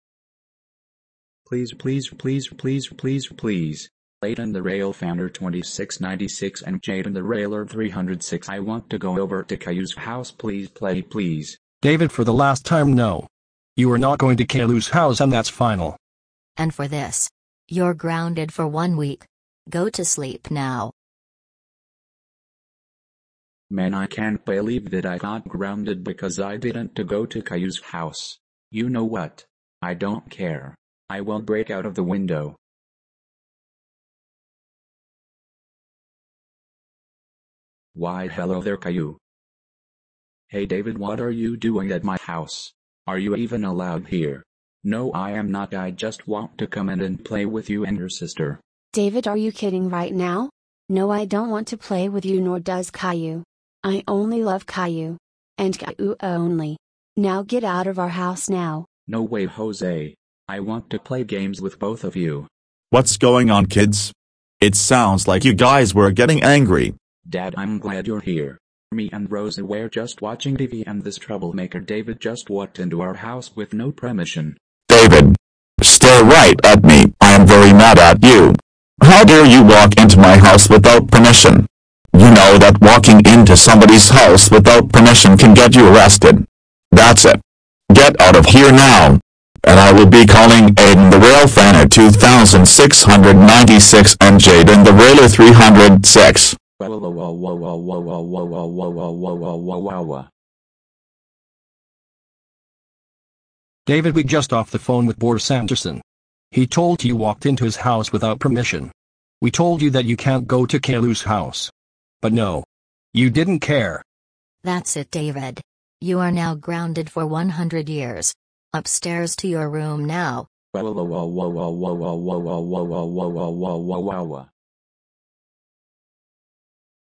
David Walks Into Caillou's House Without Permission/Grounded
And yes David Lives With Me And My Side Girl.